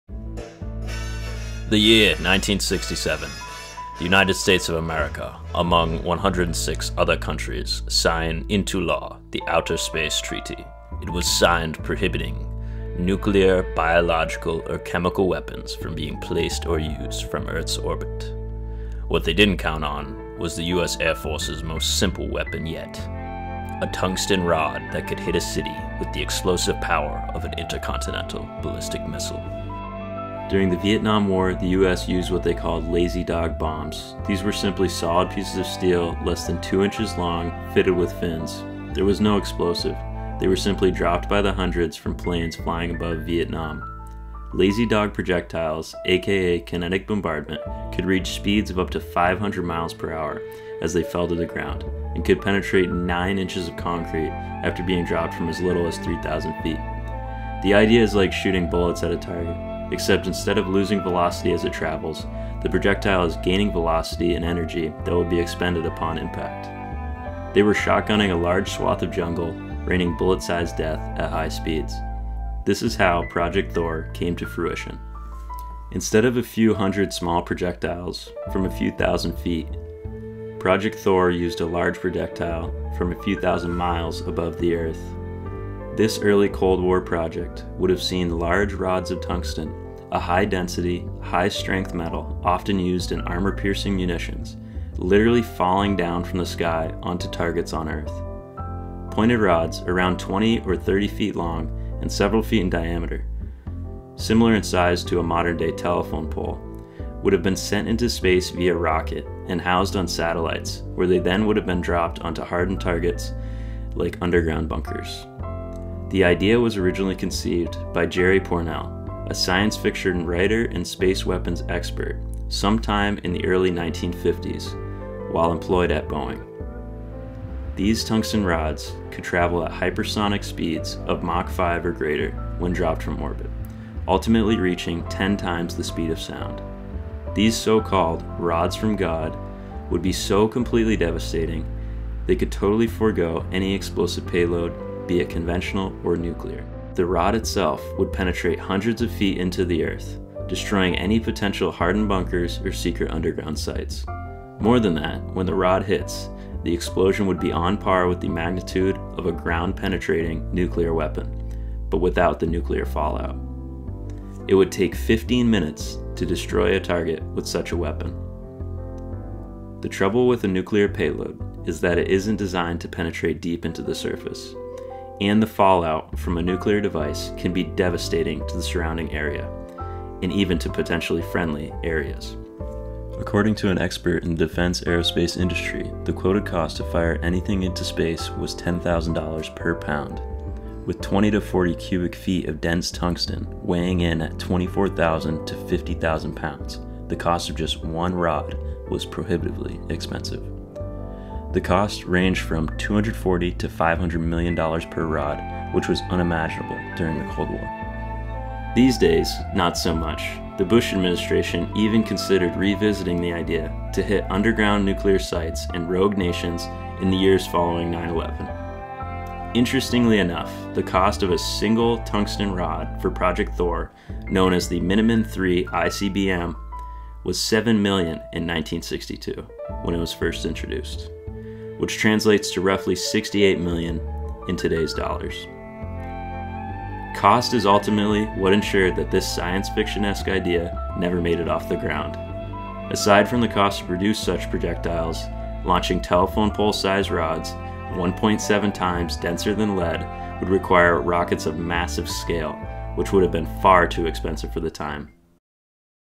CRAZIEST US SPACE WEAPON | PROJECT THOR: Rods from God
PROJECT THOR: Rods from God

In 1967 the 107-country Outer Space Treaty was signed into law prohibiting nuclear, biological, or chemical weapons from being placed or used from Earth’s orbit. What they didn’t count on was the U.S. Air Force’s most simple weapon ever: a tungsten rod that could hit a city with the explosive power of an intercontinental ballistic missile.

During the Vietnam War, the U.S. used what they called “Lazy Dog” bombs. These were simply solid steel pieces, less than two inches long, fitted with fins. There was no explosive – they were simply dropped by the hundreds from planes flying above Vietnam.

Lazy Dog projectiles (aka “kinetic bombardment”) could reach speeds of up to 500 mph as they fell to the ground and could penetrate nine inches of concrete after being dropped from as little as 3,000 feet

The idea is like shooting bullets at a target, except instead of losing velocity as it travels, the projectile is gaining velocity and energy that will be expended on impact. They were shotgunning a large swath of jungle, raining bullet-sized death at high speeds.

This is how Project Thor came to fruition.

Instead of hundreds of small projectiles from a few thousand feet, Thor used a large projectile from a few thousand miles above the Earth.

This early Cold War project would have seen large rods of tungsten, a high-density, high-strength metal often used in armor piercing munitions, literally falling down from the sky onto targets on Earth. Pointed rods, around twenty or thirty feet long and several feet in diameter, reminiscent in size of modern day telephone poles, would have been sent into space via rocket and housed on satellites, where they would then be dropped onto hardened targets like underground bunkers. The idea was conceived by Jerry Pournelle, a “science-fiction writer and space-weapons expert” sometime in the 1950s while employed at Boeing.

These tungsten rods could travel at hypersonic speeds of Mach 5 or greater  when dropped from orbit, ultimately reaching 10 times the speed of sound.  These so-called “Rods from God” would be so completely devastating, they could totally forgo any explosive payload—be it conventional, or nuclear.
The rod itself would penetrate hundreds of feet into the Earth, destroying any potential hardened bunkers or secret underground sites. More than that, when the rod hits, the explosion would be on par with the magnitude of a ground-penetrating nuclear weapon – but with no fallout.
It would take 15 minutes to destroy a target with such a weapon.

The trouble with a nuclear payload is that it isn’t designed to penetrate deep into the surface. And the fallout from a nuclear device can be devastating to surrounding, potentially friendly areas.

According to an expert in the defense aerospace industry the quoted cost to fire anything into space was $10,000 per pound. With 20 – 40 cubic feet of dense tungsten weighing in at just over 24,000 – 50,000 pounds, the cost of just one rod was prohibitively expensive. The cost of $240  to $500 million dollars per rod was unimaginable during the cold war.

These days, not so much. The Bush Administration even considered revisiting the idea to hit underground nuclear sites in rogue nations in the years following 9/11. Interestingly enough, the cost of a single Minuteman III ICBM was $7 million in 1962, when it was first introduced (which is $68 million when adjusted for inflation).

Cost is what ensured that this science fiction-esque idea never made it off the ground. Aside from the cost to produce such projectiles, launching telephone pole sized rods, 1.7 times denser than lead – would require rockets of massive scale that would have been prohibitively expensive.